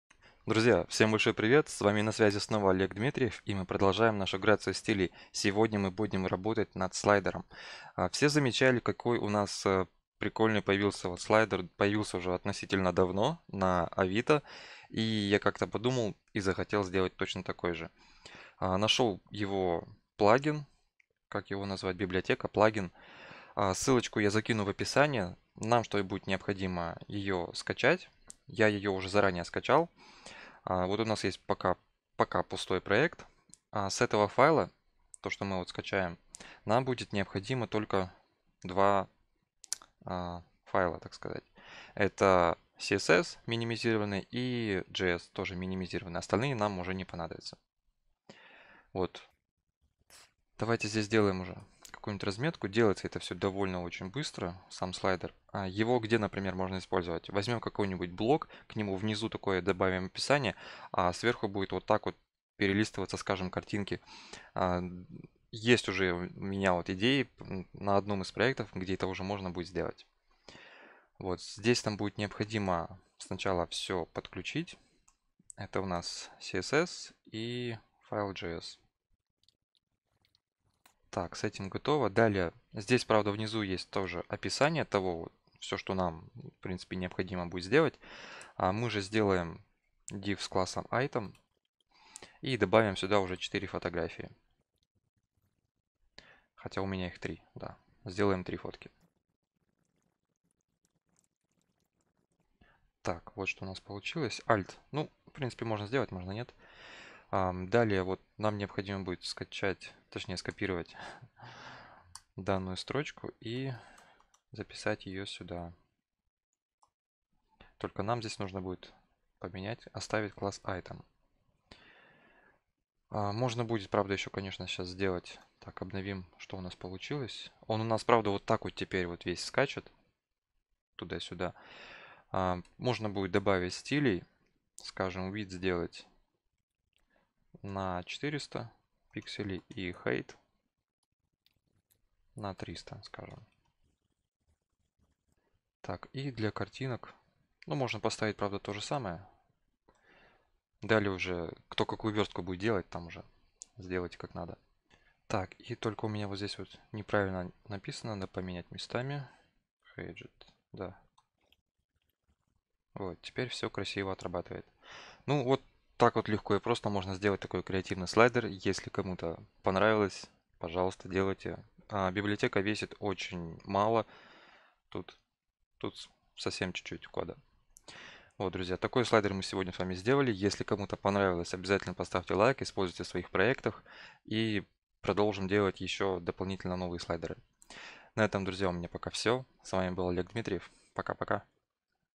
Грация CSS #2 ➤ Slider - прям как на Авито
В данном видео мы будем делать слайдер прямо как на Авито, будет классно))

Так же можно заказать консультацию по заработку на фрилансе либо по веб-разработке